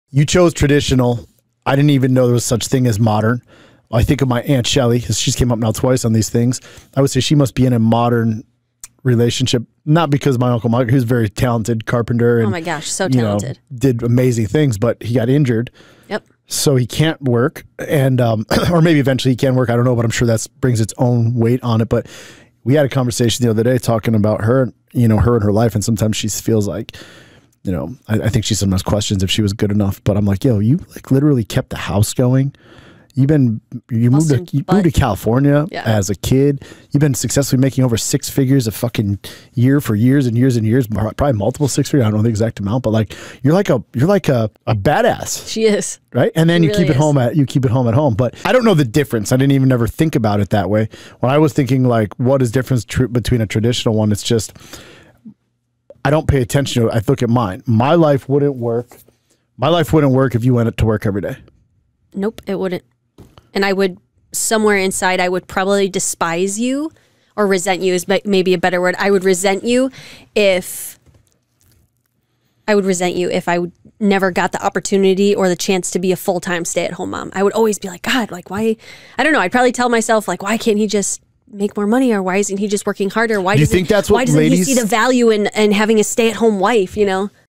TRADITIONAL RELATIONSHIP
Despite what the various groups would say... Traditional relationships work for a reason. If it's not for you.. cool move along, but for many its the only way to thrive.